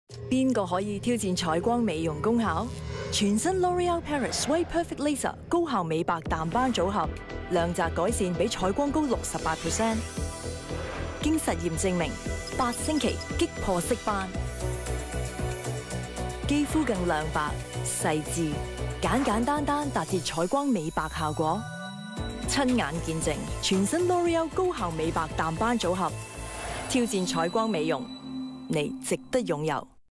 L'OREAL WHITE PERFECT LASER 高效美白淡斑組合 - 范冰冰
WHITE PERFECT LASER
高效美白淡斑組合
祛斑亮白精華 + 美白防護日霜
L'OREAL PARiS
丘凱敏旁白